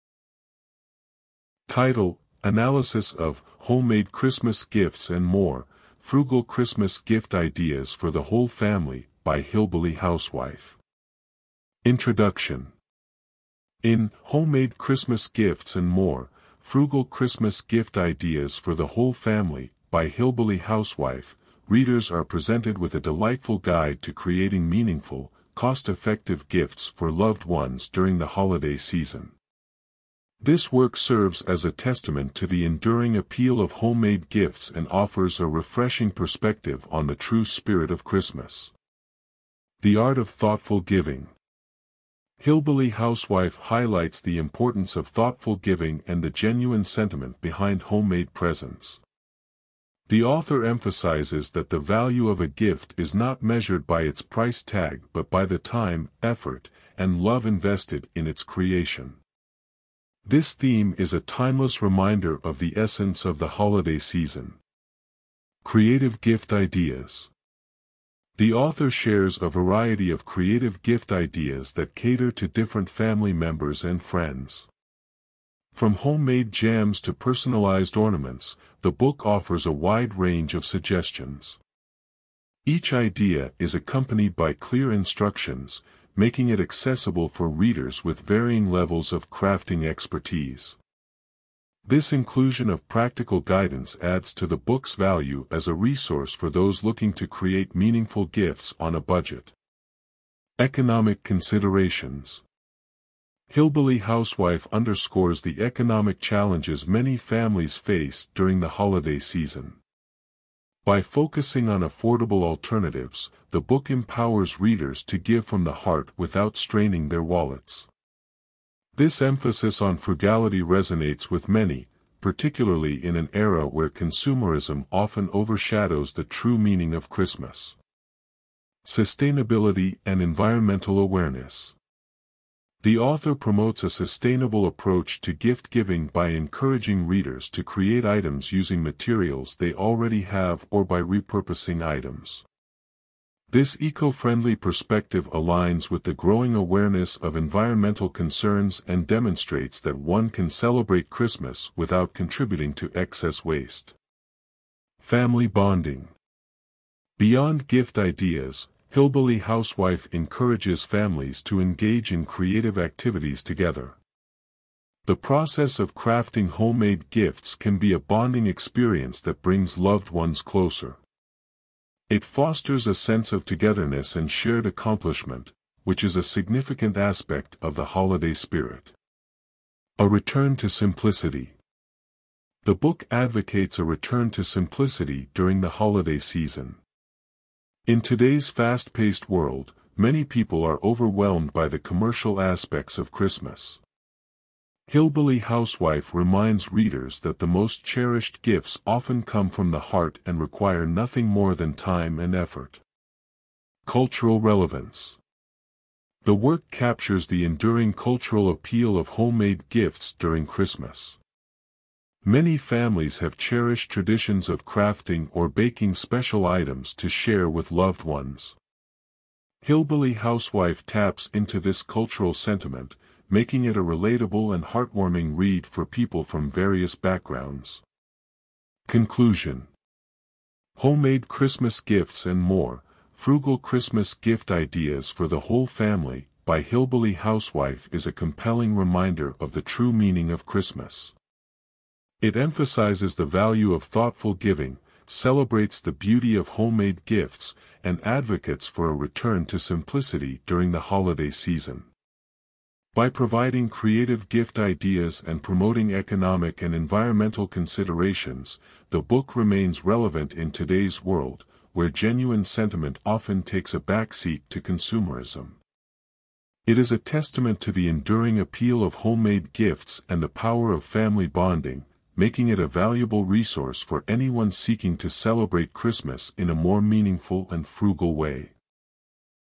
"Homemade Christmas Gifts and More" By Hillbilly Housewife
In "Homemade Christmas Gifts and More - Frugal Christmas Gift Ideas For The Whole Family" by Hillbilly Housewife, readers are presented with a delightful guide to creating meaningful, cost-effective gifts for loved ones during the holiday season. This work serves as a testament to the enduring appeal of homemade gifts and offers a refreshing perspective on the true spirit of Christmas.

The Art of Thoughtful Giving:
Hillbilly Housewife highlights the importance of thoughtful giving and the genuine sentiment behind homemade presents. The author emphasizes that the value of a gift is not measured by its price tag but by the time, effort, and love invested in its creation. This theme is a timeless reminder of the essence of the holiday season.

Creative Gift Ideas:
The author shares a variety of creative gift ideas that cater to different family members and friends. From homemade jams to personalized ornaments, the book offers a wide range of suggestions. Each idea is accompanied by clear instructions, making it accessible for readers with varying levels of crafting expertise. This inclusion of practical guidance adds to the book's value as a resource for those looking to create meaningful gifts on a budget.

Economic Considerations:
Hillbilly Housewife underscores the economic challenges many families face during the holiday season. By focusing on affordable alternatives, the book empowers readers to give from the heart without straining their wallets. This emphasis on frugality resonates with many, particularly in an era where consumerism often overshadows the true meaning of Christmas.

Sustainability and Environmental Awareness:
The author promotes a sustainable approach to gift-giving by encouraging readers to create items using materials they already have or by repurposing items. This eco-friendly perspective aligns with the growing awareness of environmental concerns and demonstrates that one can celebrate Christmas without contributing to excess waste.

Family Bonding:
Beyond gift ideas, Hillbilly Housewife encourages families to engage in creative activities together. The process of crafting homemade gifts can be a bonding experience that brings loved ones closer. It fosters a sense of togetherness and shared accomplishment, which is a significant aspect of the holiday spirit.

A Return to Simplicity:
The book advocates a return to simplicity during the holiday season. In today's fast-paced world, many people are overwhelmed by the commercial aspects of Christmas. Hillbilly Housewife reminds readers that the most cherished gifts often come from the heart and require nothing more than time and effort.

Cultural Relevance:
The work captures the enduring cultural appeal of homemade gifts during Christmas. Many families have cherished traditions of crafting or baking special items to share with loved ones. Hillbilly Housewife taps into this cultural sentiment, making it a relatable and heartwarming read for people from various backgrounds.

Conclusion:
"Homemade Christmas Gifts and More - Frugal Christmas Gift Ideas For The Whole Family" by Hillbilly Housewife is a compelling reminder of the true meaning of Christmas. It emphasizes the value of thoughtful giving, celebrates the beauty of homemade gifts, and advocates for a return to simplicity during the holiday season. By providing creative gift ideas and promoting economic and environmental considerations, the book remains relevant in today's world, where genuine sentiment often takes a backseat to consumerism. It is a testament to the enduring appeal of homemade gifts and the power of family bonding, making it a valuable resource for anyone seeking to celebrate Christmas in a more meaningful and frugal way.